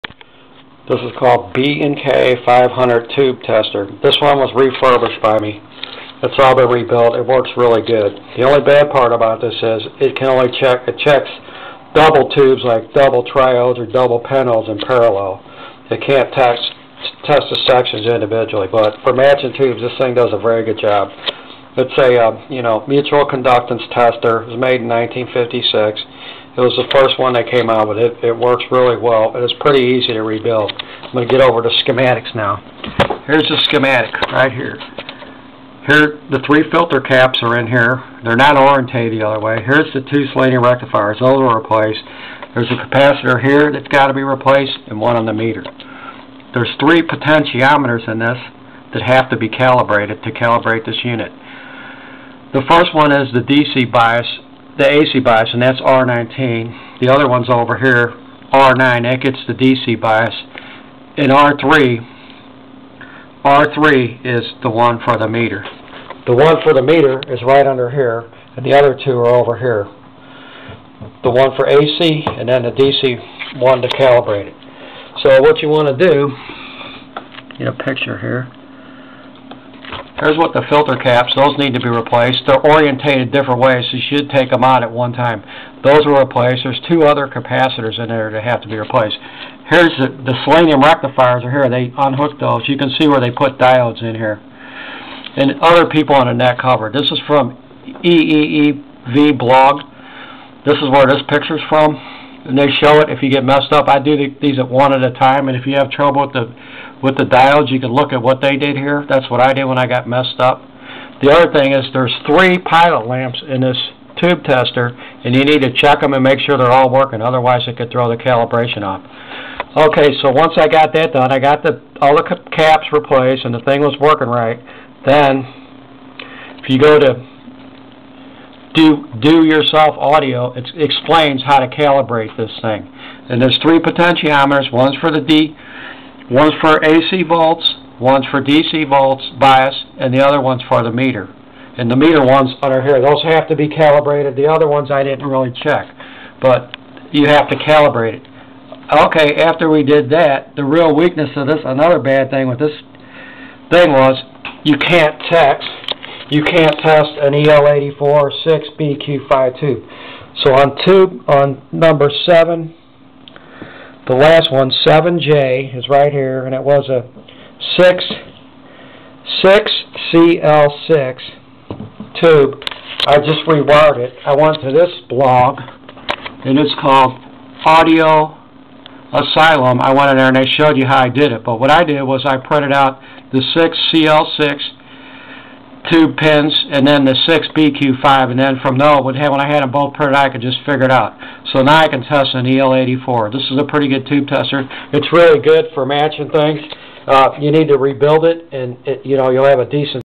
B&K 500 Tube Tester Refurbished.
B&K 500  Tube Tester, it has 5 new capacitors in it. The Selenium Rectifiers were replaced with Silicone Diodes. The three potentiometers ac bias, dc bias and the meter were calibrated. The 6cl6 socket was rewired for 6bq5 tube. EEV blog shows how the filter caps and the diodes are to be installed.  Diyaudio has calibration instruction and  Tubes Asylum has how to rewire the tune socket for a 6bq5 tube. Also be sure to check your heater voltages.